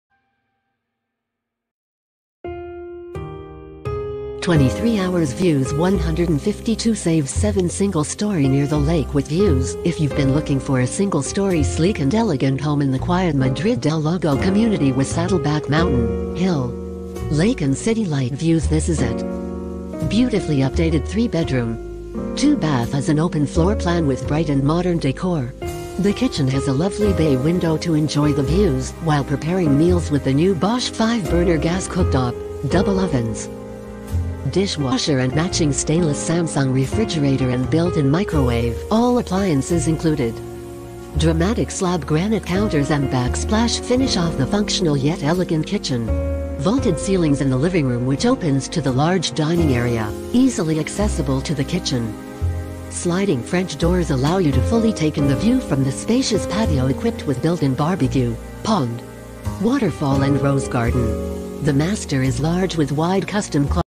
Summary - Mission Viejo, , CA 92691 Home Sale
23 hours Views 152 Saves 7 Single story near the lake with viewsIf you've been looking for a single story sleek and elegant home in the quiet Madrid del Lago community with Saddleback mountain, hill, lake and city light views - this is it! Beautifully updated 3 bedroom, 2 bath has an open floor plan with bright and modern decor.The kitchen has a lovely bay window to enjoy the views while preparing meals with the new Bosch 5 burner gas cooktop, double ovens, dishwasher and matching stainless Samsung refrigerator and built-in microwave (all appliances included). Dramatic slab granite counters and backsplash finish off the functional yet elegant kitchen.Vaulted ceilings in the living room which opens to the large dining area, easily accessible to the kitchen. Sliding French doors allow you to fully take in the view from the spacious patio equipped with built-in barbecue, pond, waterfall and rose garden.The master is large with wide custom closet complete with 8 foot mirrored doors. The master bath has rectangular sinks, granite counters and oil rubbed bronze fixtures. What I love about this home The views. During the day you can see blue skies, lots of greenery, roses and majestic mountains in the distance. And with the open floorplan you can enjoy the views from every room. With the front-loaded street design there are no immediate neighbors visible from the front of the house. By night, lounging on the back patio near the barbecue, pond and waterfall, the city lights are beautiful and calming, especially since its a very quiet community.Love the ultra low maintenance of the house. The front lawn synthetic turf and satio tile backyard are maintenance-free. Yet there are areas to grow plants and over a dozen mature roses. I also enjoy the lighting in the house with stylish fixtures that illuminate for working or elegant atmosphere.Oh, and I love the garage! There's an epoxy garage floor coating that is so easy to maintain and always looks great. There's a 16 foot workbench (included) that let's me spread out to work on multiple projects. Neighborhood home value One-year prediction Median home comparison $731,800

Mission Viejo,  Real Estate Sale
Mission Viejo,  Property Sale
Mission Viejo,  Homes Sale
Mission Viejo,  Single_Family Sale